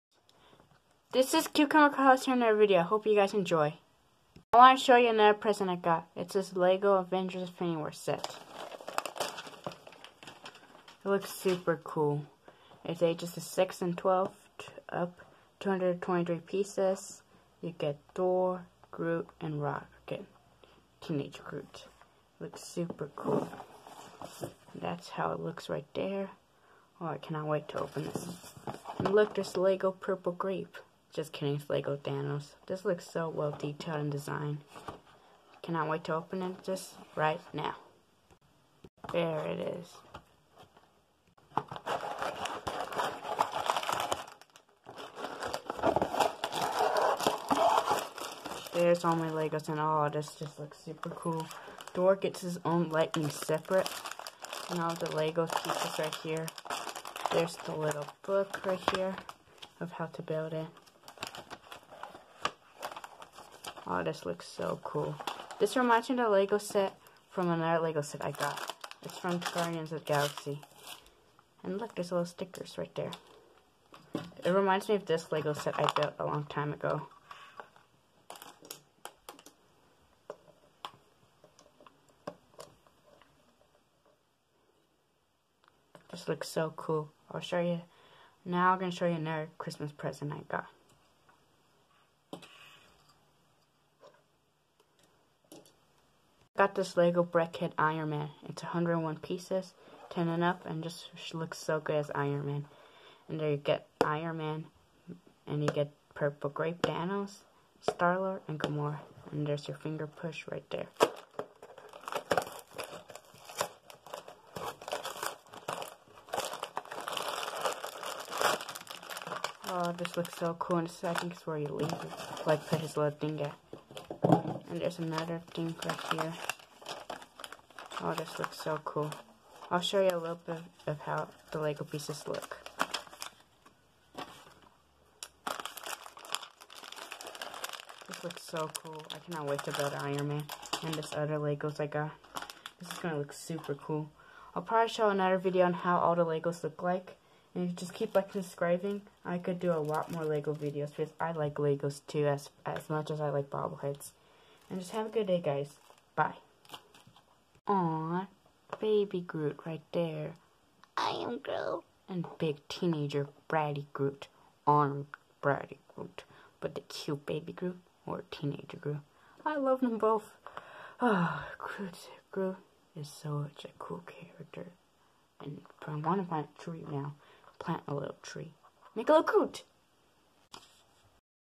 Showing my Christmas presents
Today I will be showing you my other Christmas presents.